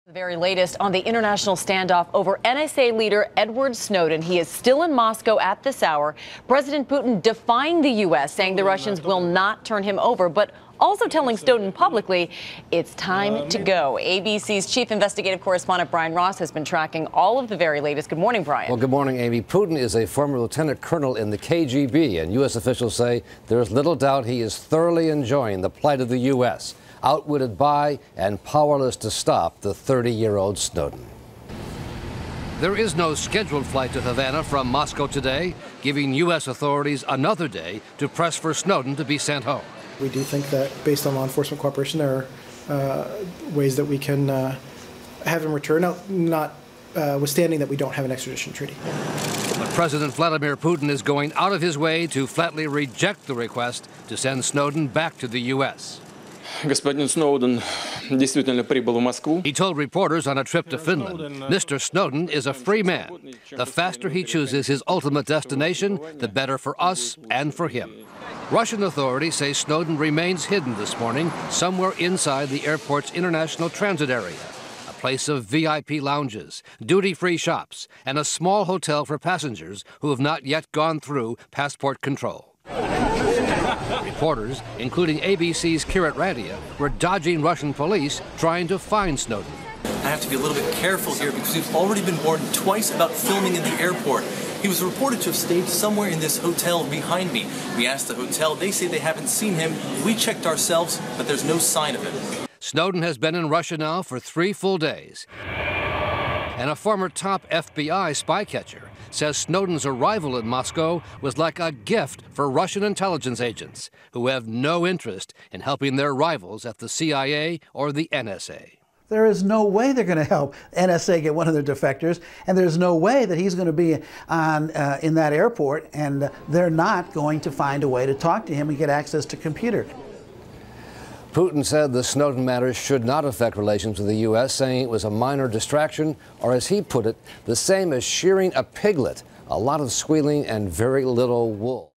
Edward Snowden NSA Leaks: Putin Won't Turn Snowden Over, Wants Him Out
Russian president says the NSA leaker is a free man, but made it clear it's time to go.